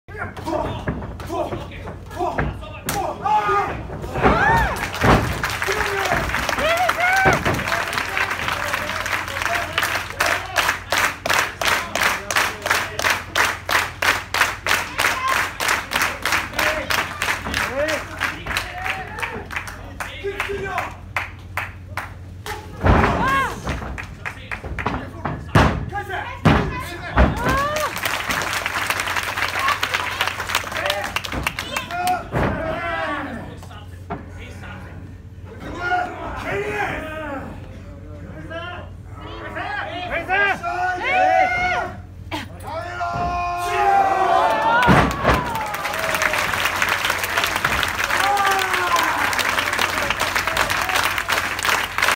宮本裕向が美しいキックのコンビネーションをクリス・ブルックスに決める。タッチを受けた木高イサミのトップロープからのダイビングボディアタック 我闘雲舞「Phoenix Rises」 2022年9月15日
我闘雲舞「Phoenix Rises」 2022年9月15日

セミファイナル アジアドリームタッグ選手権試合 30分一本勝負
高梨将弘、クリス・ブルックス VS 木高イサミ、宮本裕向

我闘雲舞の試合会場での動画撮影・公開は1分前後なら可能となっています。(試合会場でのアナウンスにて確認)
当該ポリシーに従って全編動画の場合は1分前後、一部利用の場合は動画の時間が1分以内になるように編集しています。